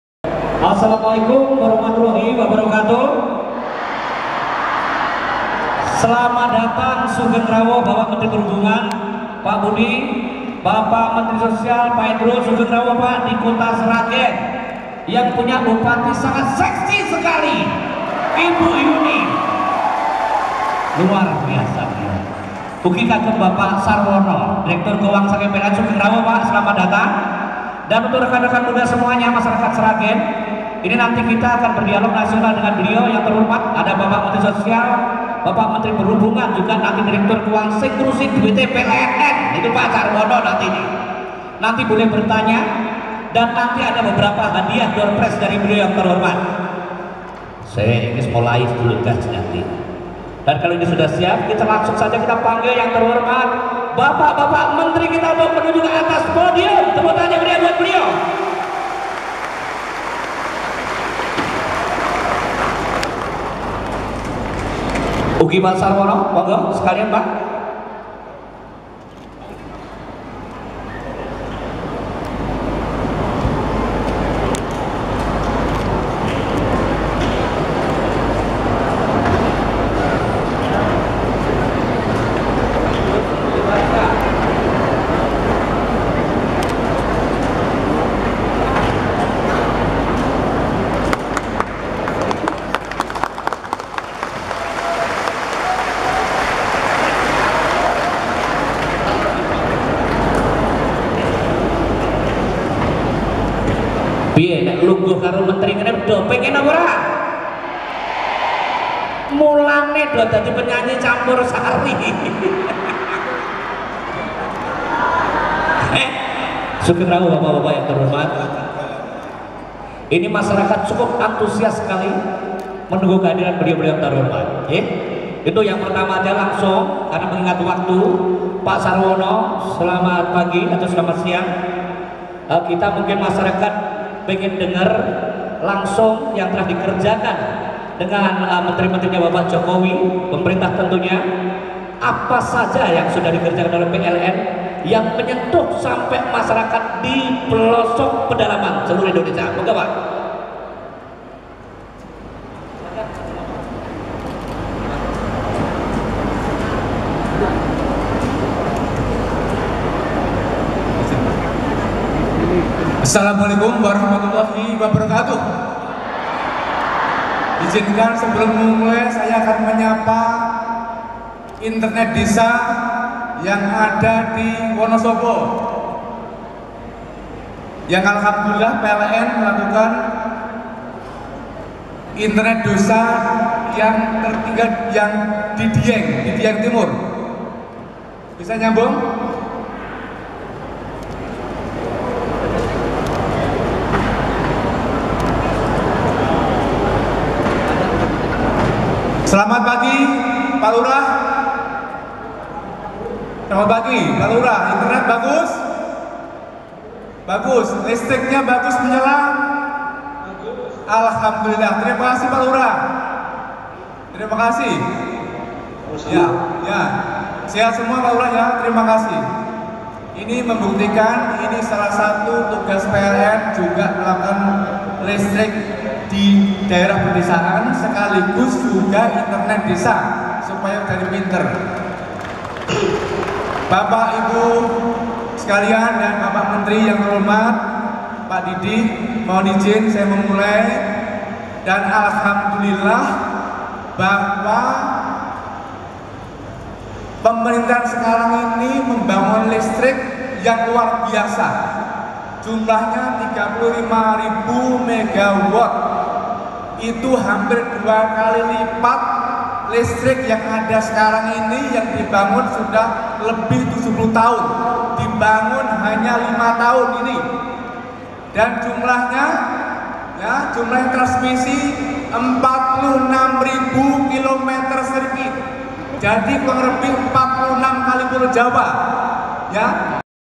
Didi Kempot Jadi Moderator Dialog Nasional 9 Indonesia Maju
Pada Sabtu 31 Maret 2018 Pukul 09:00 Wib
MENTERI SOSIAL REPUBLIK INDONESIA  M.Idrus Marham
Menteri Perhubungan Republik Indonesia : Budi Karya Sumadi.
Derut PLN Republik Indonesia : Sofyan Basir
Bupati Sragen Kusdinar Untung Yuni Sukowati
Wakil Bupati Sragen Dedy Endriyatno
Kapolres Sragen AKBP Arif Budiman
Dandim 0725 Sragen Arh Camas Sigit Prasetyo

Bertempat Di Gor Diponegoro Kabupaten Sragen
Jalan Raya Sukowati Sragen, provinsi Jawa Tengah , Indonesia
Bintang Tamu / Artis Indonesia
Judika
2 Racun, Dua Racun
Didi Kempot